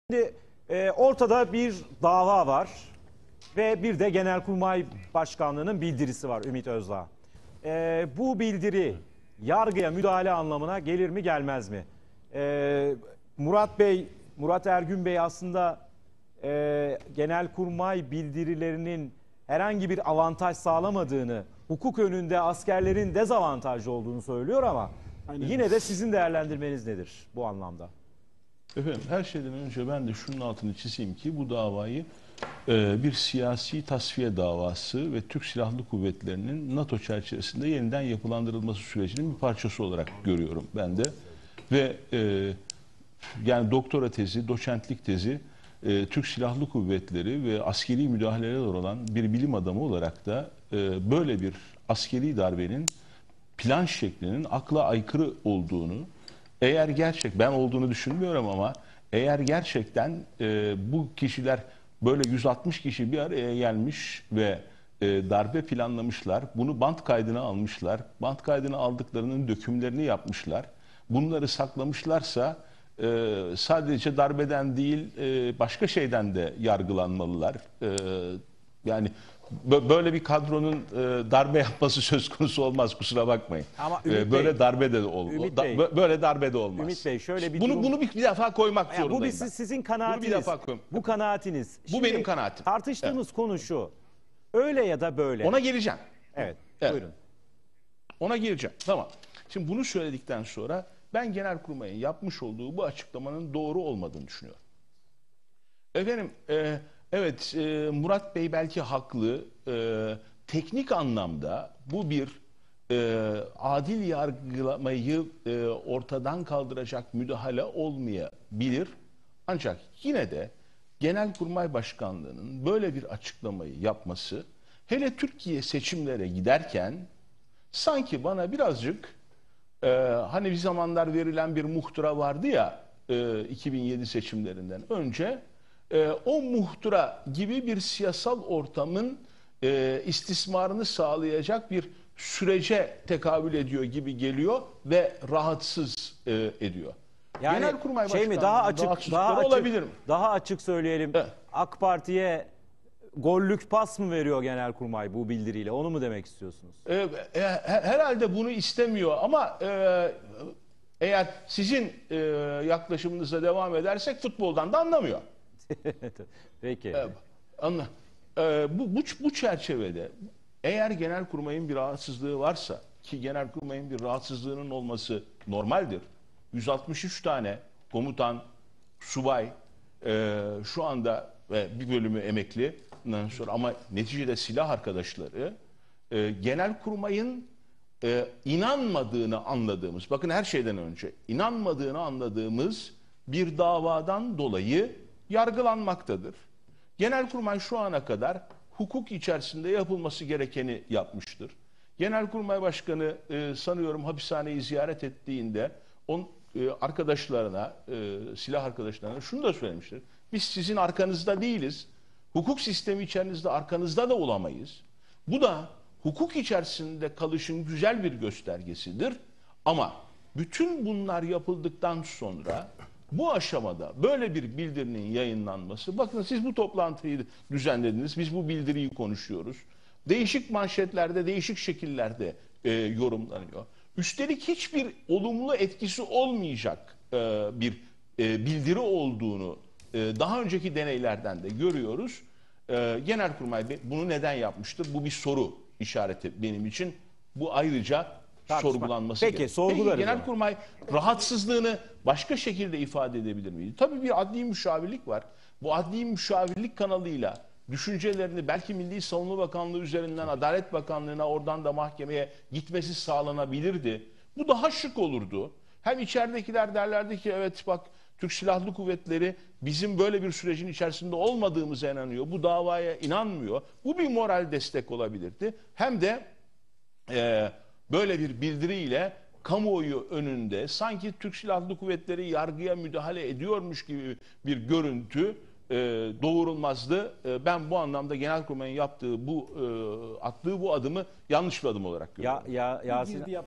Ümit Özdağ'dan Balyoz Davası Hakkında Yorumlar
Genelkurmay yaptığı Balyoz çıkışı ile AKP'ye gollük pas mı verdi?

8 Nisan 2011 - CNN Türk